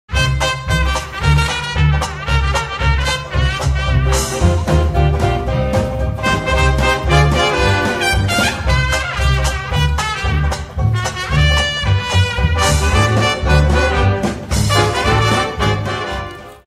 Soccer
A funny movie about Conrad and Allegra playing soccer.